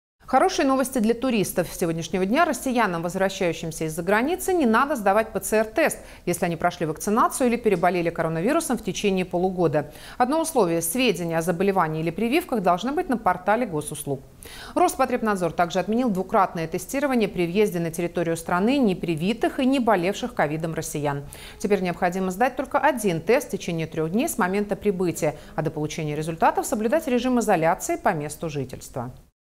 Россиян будут пускать на родину без ПЦР-теста
Хорошие новости для туристов. С сегодняшнего дня россиянам, возвращающимся из-за границы, не надо сдавать ПЦР-тест, если они прошли вакцинацию или переболели коронавирусом в течение полугода. Одно условие - сведения о заболевании или прививках должны быть на портале госуслуг. Роспотребнадзор также отменил двукратное тестирование при въезде на территорию страны непривитых и неболевших ковидом россиян. Теперь необходимо сдать только один тест в течение трех дней с момента прибытия. А до получения результатов соблюдать режим изоляции по месту жительства.